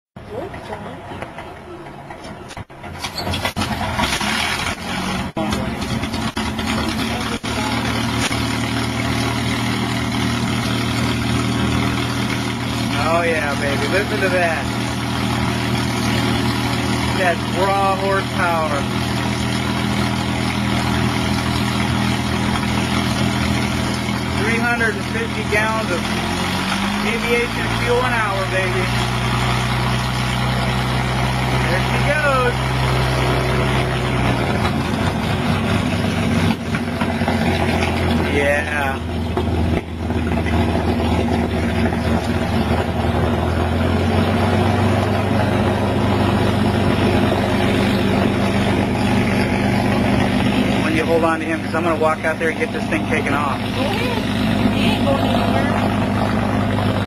P51 Mustang Starting Engine Ukiah Airport
P51 Mustang starting its engine and getting ready to takeoff